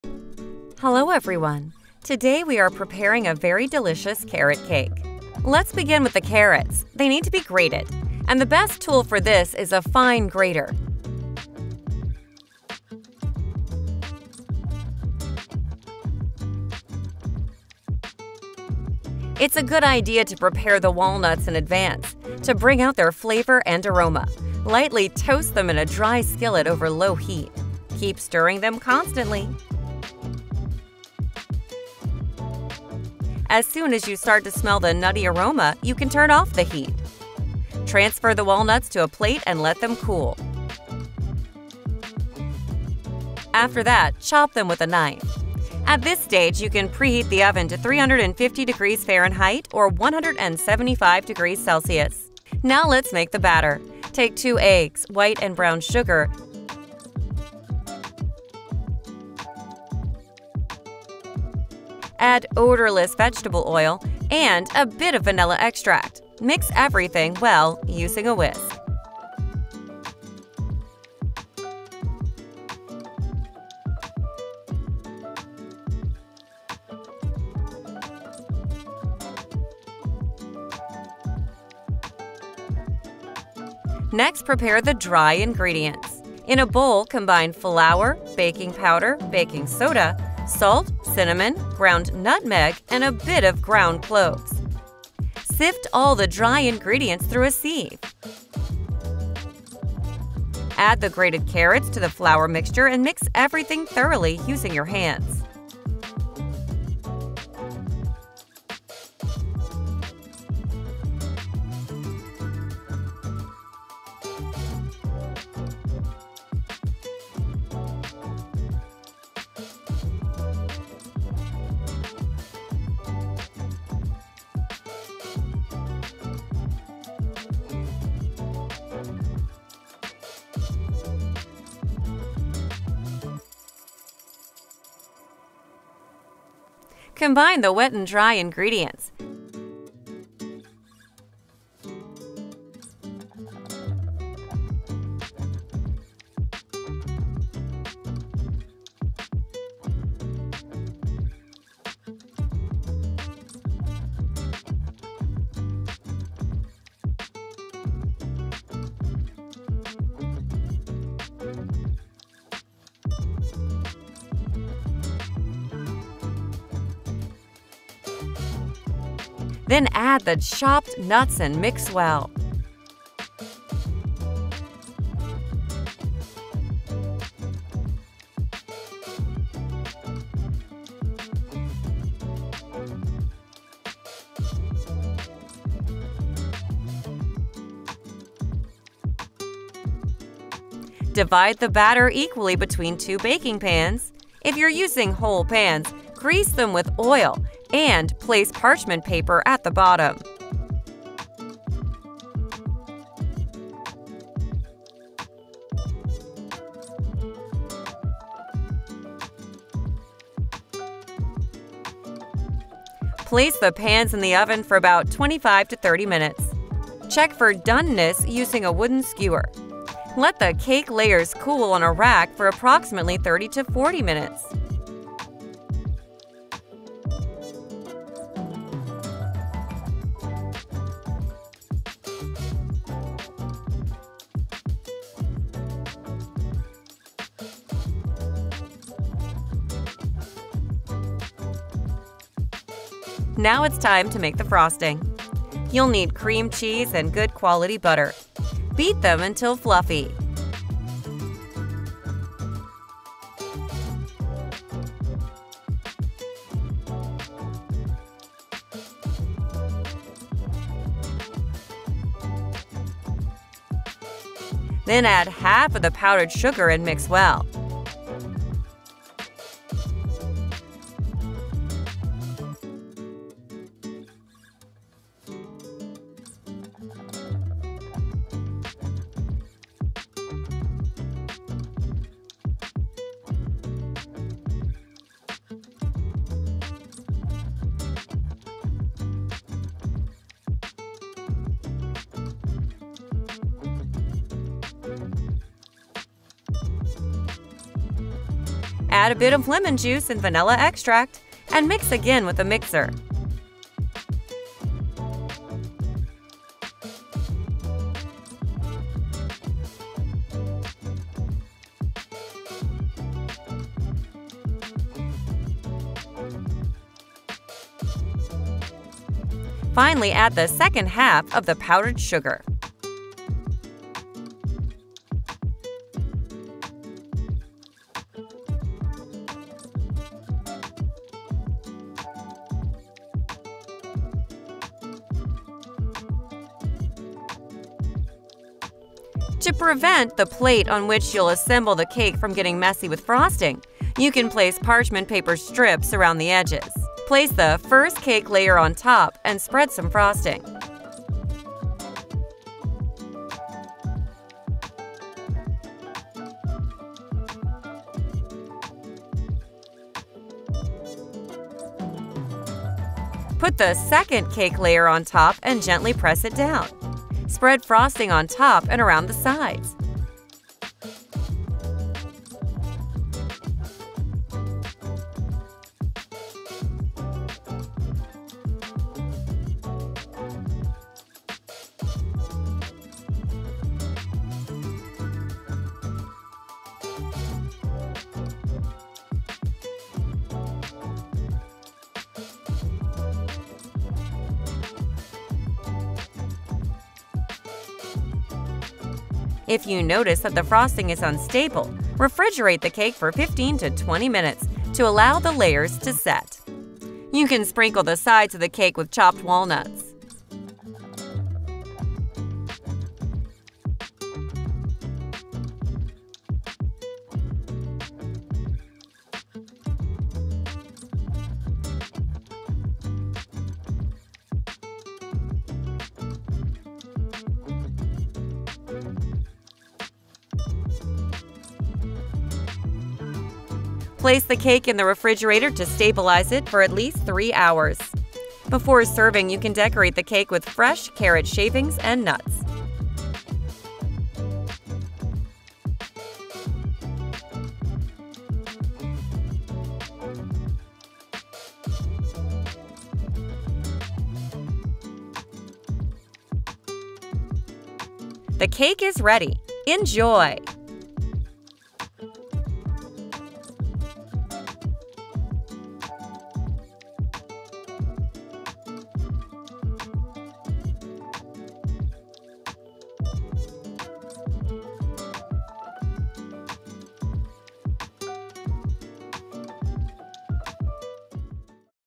Bake the carrot cake in 5 minutes! Everyone is looking for this recipe! Simple and tasty
☺️ Hello, friends! How's your mood? Today, I tried making a carrot cake; it's a delicate and delicious dessert. If you have carrots, 2 eggs, and flour, you can make this tasty carrot-flavored cake.

For this simple and tasty recipe, you will need:

½ cup | 115 g vegetable oil
2 eggs
⅔ cup | 135 g light brown sugar
⅓ cup | 65 g granulated sugar
½ tsp vanilla extract
1 ½ cups | 200 g all-purpose flour
1.5 tsp baking powder
½ tsp baking soda
½ tsp ground cinnamon
½ tsp salt
¼ tsp ground allspice
¼ tsp ground cloves
1 cup | 140 g finely grated carrots
½ cup | 50 g lightly toasted walnuts or raisins (optional)

¾ cup | 175 g cream cheese
¼ cup | 70 g unsalted butter temperature
2 ⅛ cups | 260 g sifted powdered sugar
1 tsp vanilla extract
1 tsp lemon juice
½ cup | 50 g lightly toasted walnut pieces